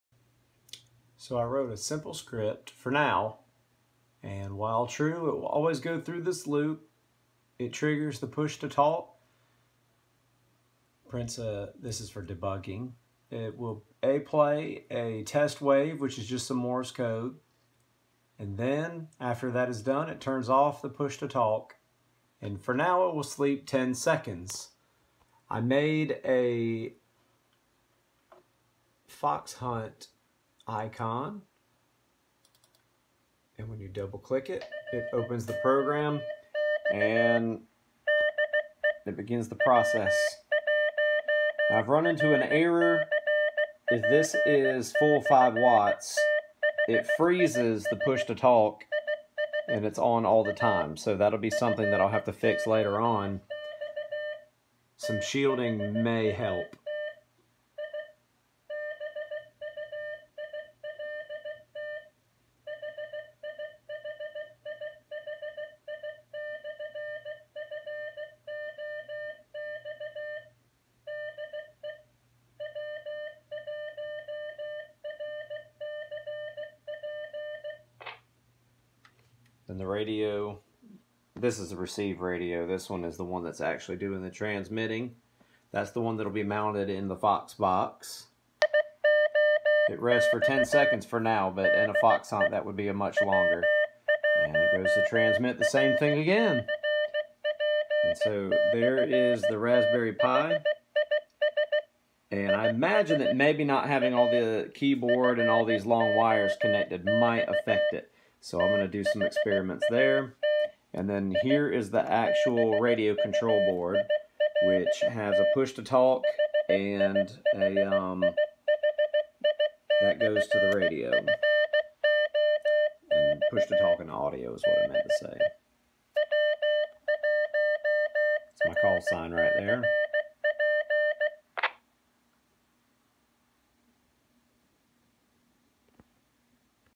Raspberry Pi Controlled Amateur Radio Fox Transmitter
- Using what had at home I created a Raspberry Pi controlled FT-60 fox transmitter. Of course, Santa Claus is bringing me the power supply. This video is proof of a working prototype. I wrote a short Python script to play an audio file of CW through the FT-60.